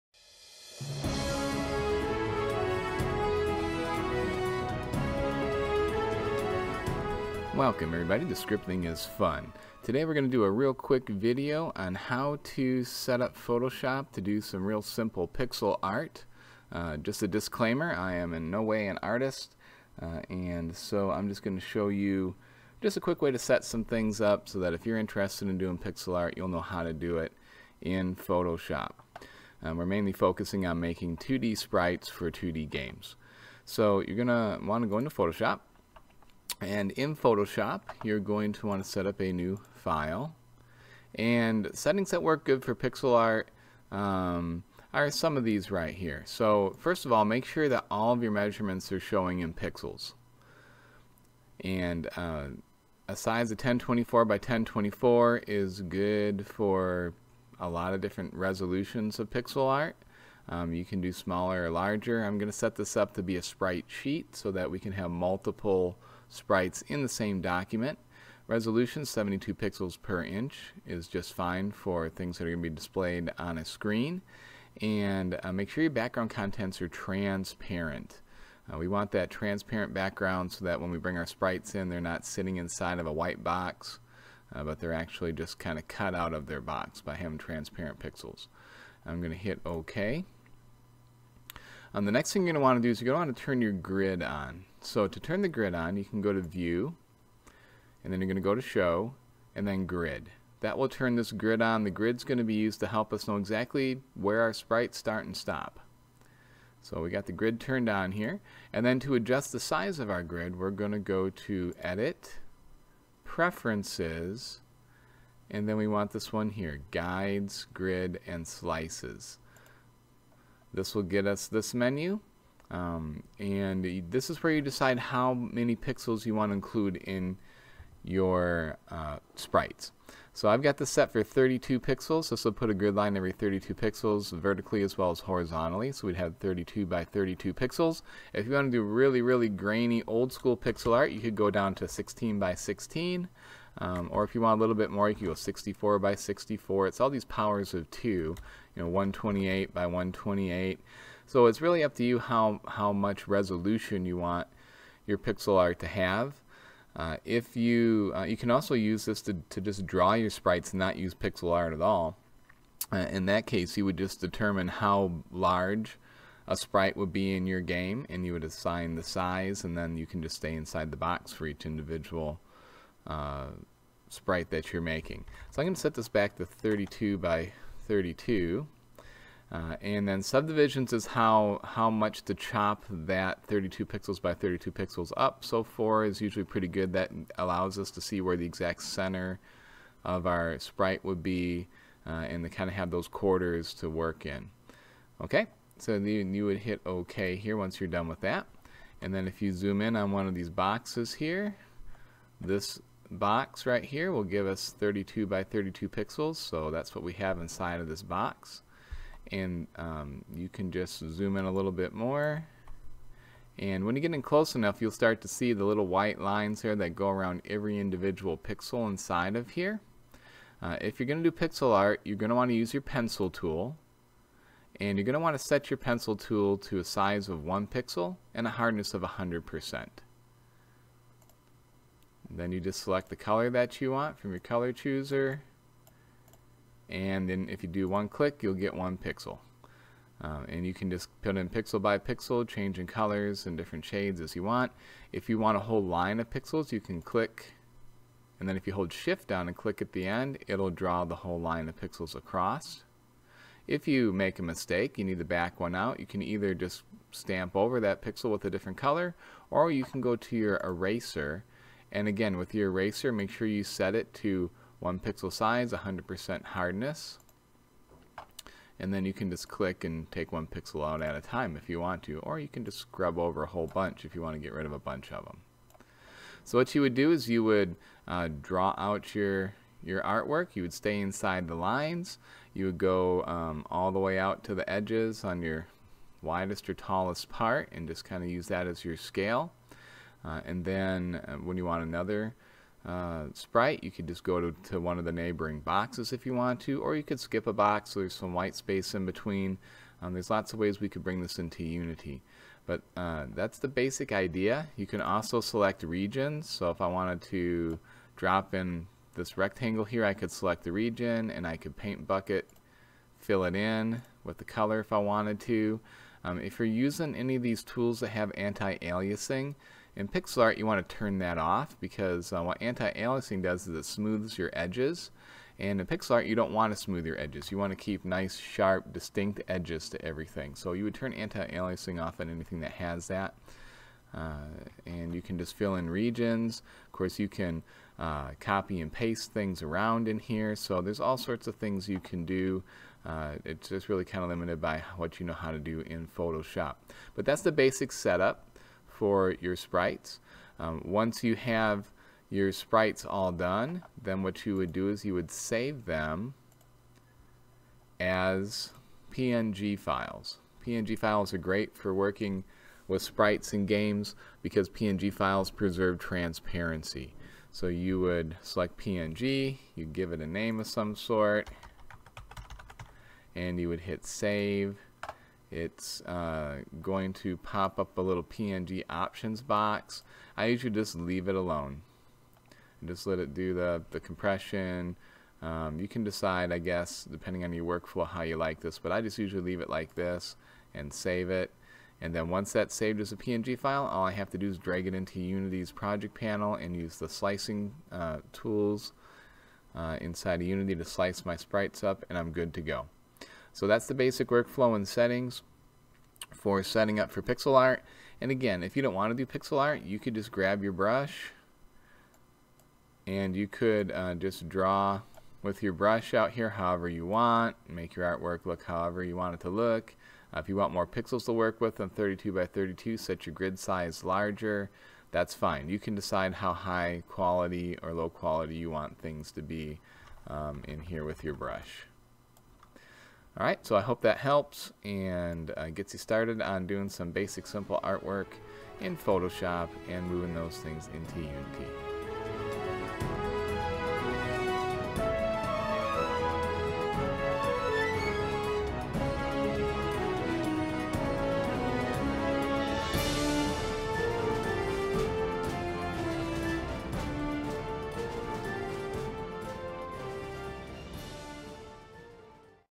How to Setup PhotoShop for Making Sprites
This is a quick video on one way to setup PhotoShop to make 2D pixel art sprites for games.